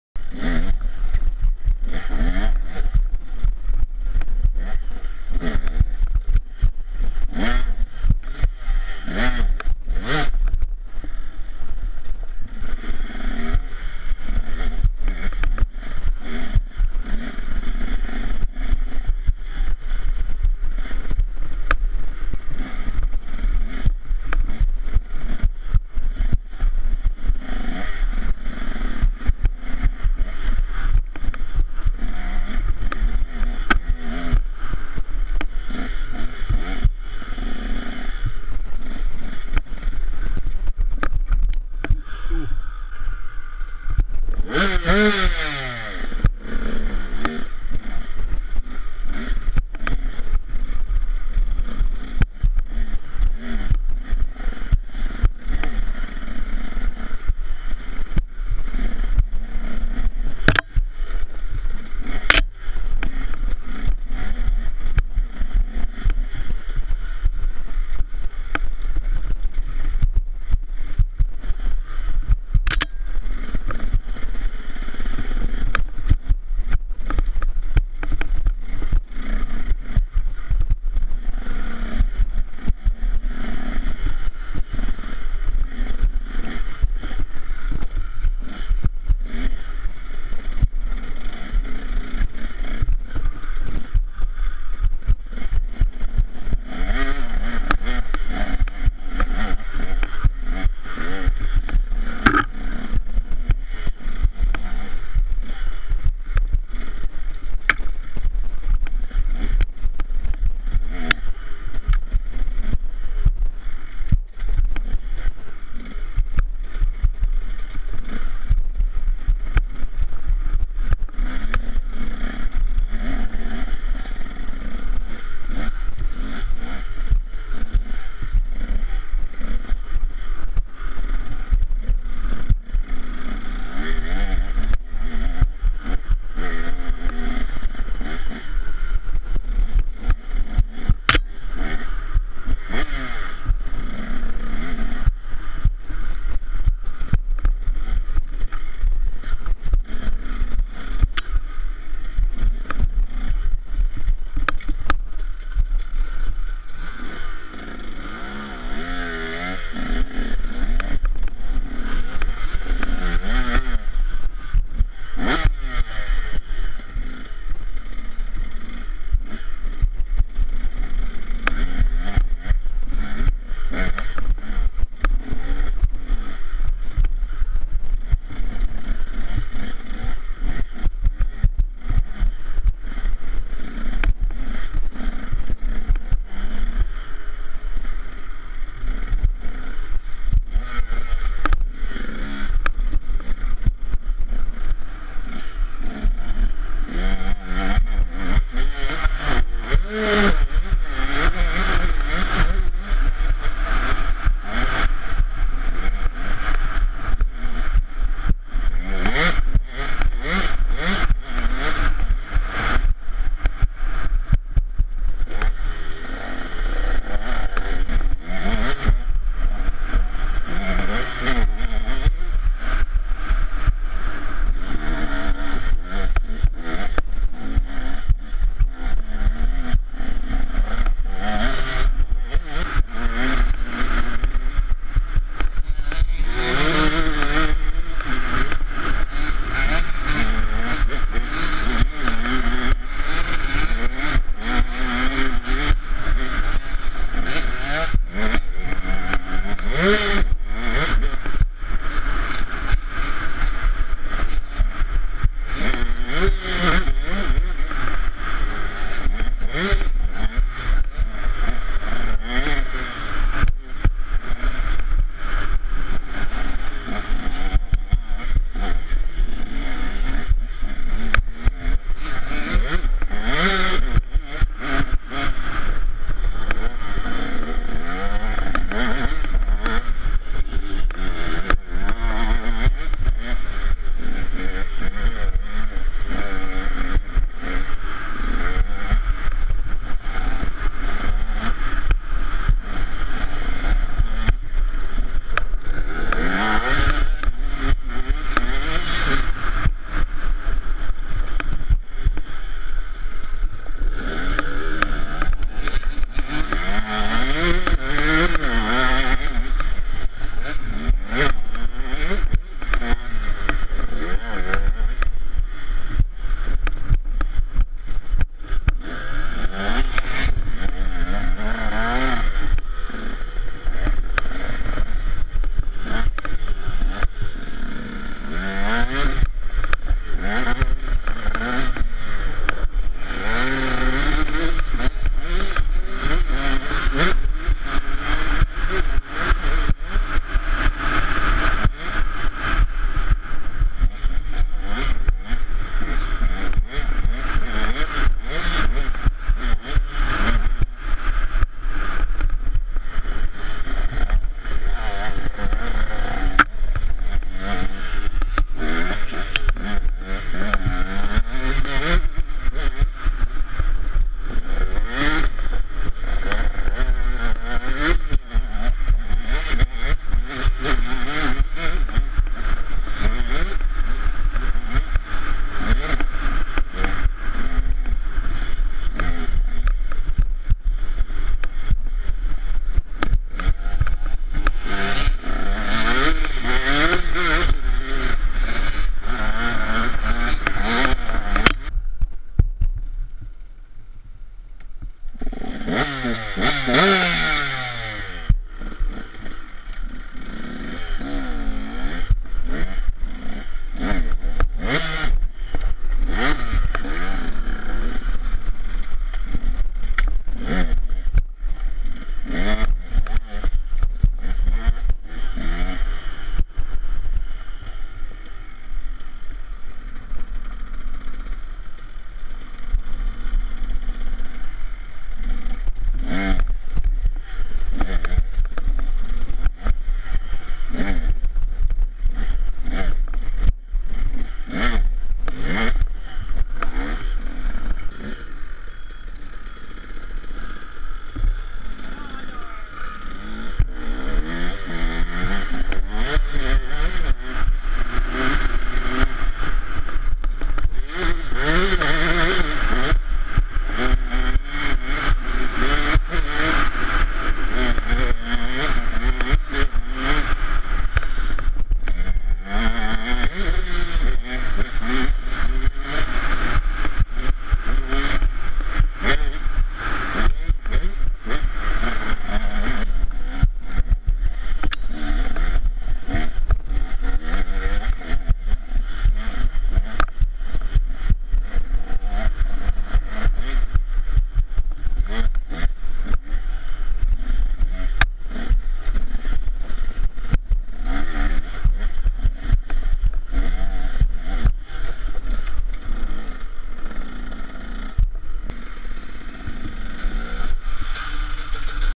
Näkkilä Racing Rocks logs and really fast trail
paulaniemessä kivikkoo,nopeempaa reittii ja extreme rataa